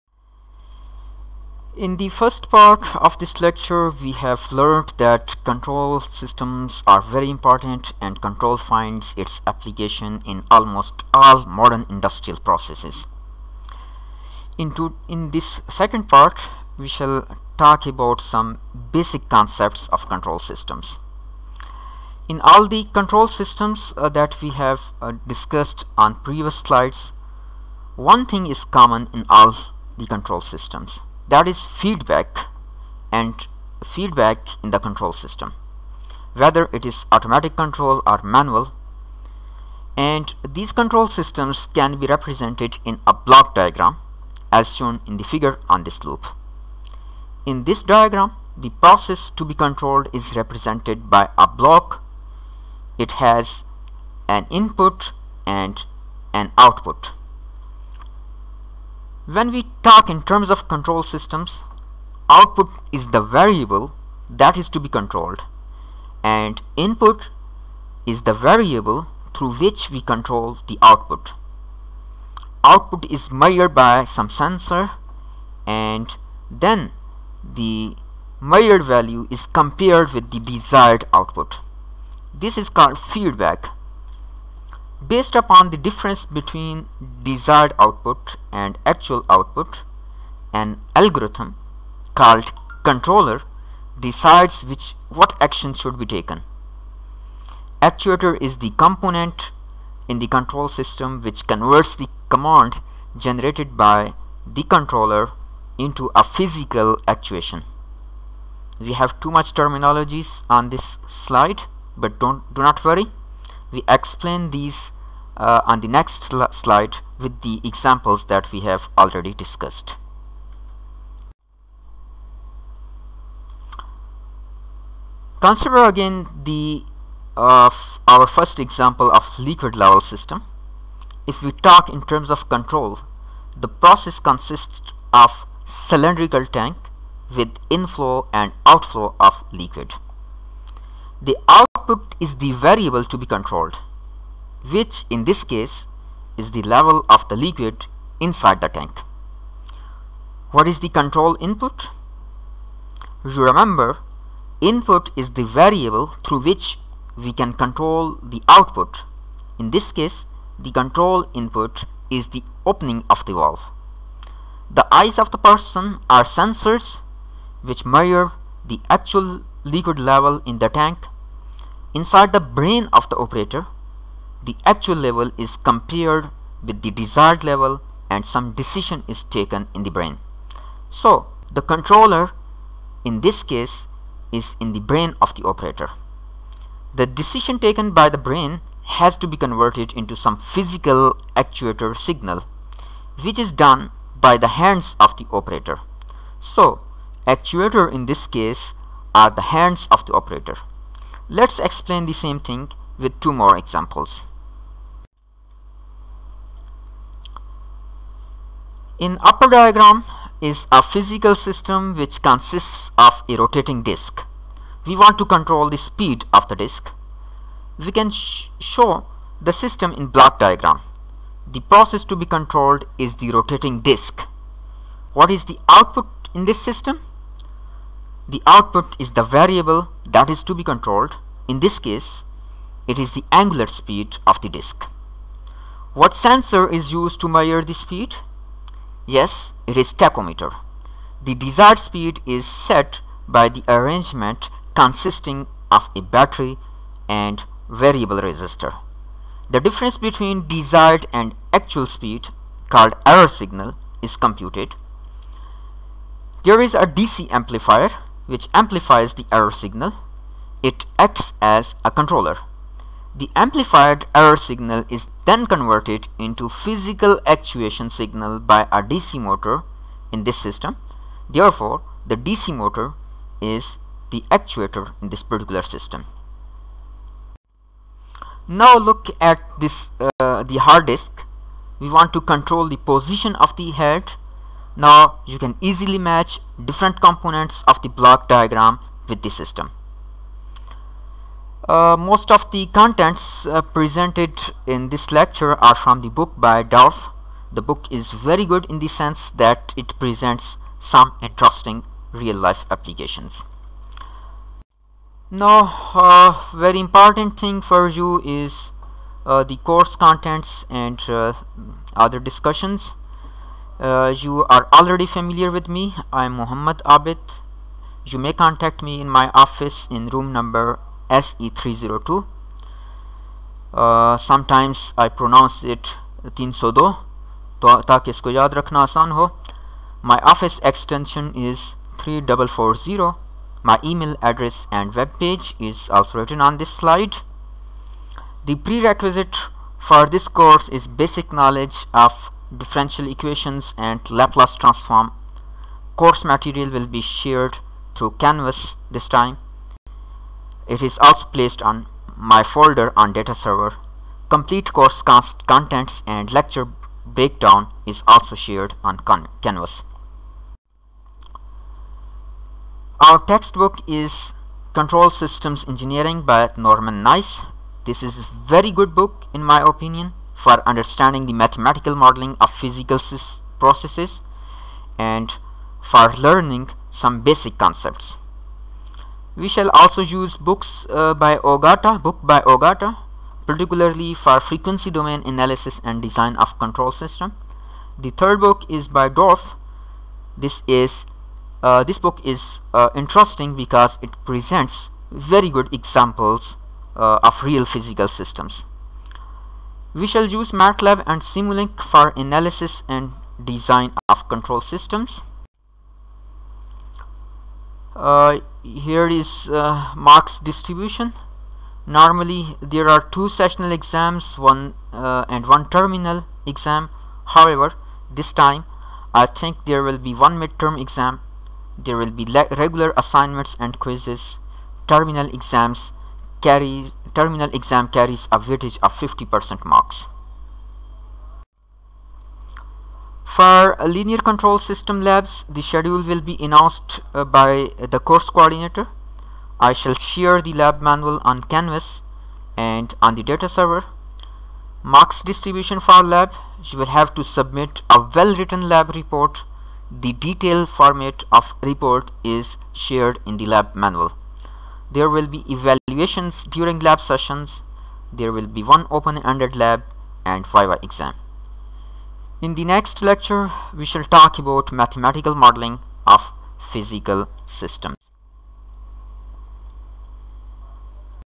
LCS 1b - Feedback control and control loop
Course Title: Linear Control Systems

This is my second video lecture on Linear Control Systems offered at PIEAS to 7th semester students. The lecture describes the importance of control systems, some brief historical background and some applications in modern industrial processes. Most of the contents in this lecture are from the book by Dorf.